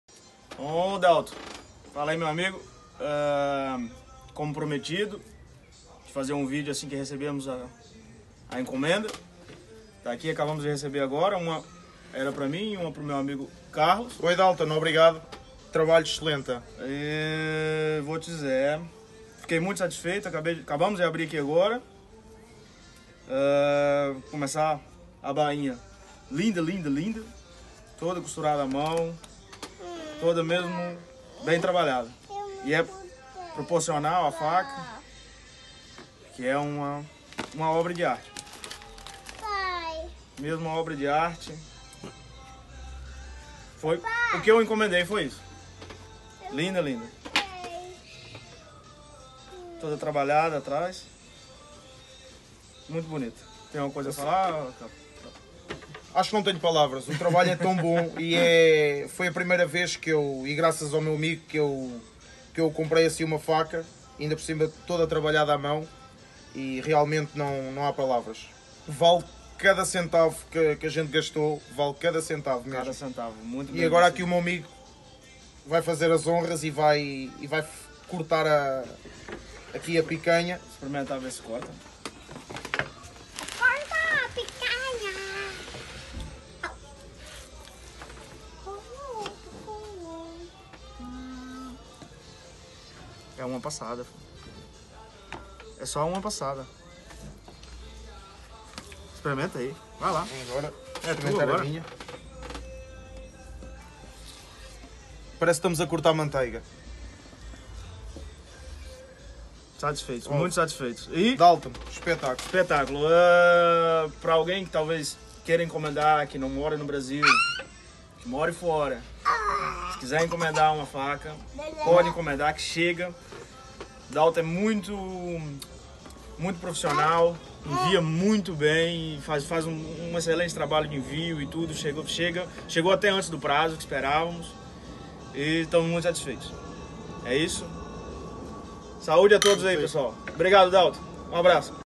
Facas em mola de trem feitas por encomenda para RENATO AREIA de GROSLAY / FRANÇA
Facas feitas  em molande trem (aço 5160 ) por encomendas para RENATO AREIA

dúvidas pelo watts no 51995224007com DALTON FACAS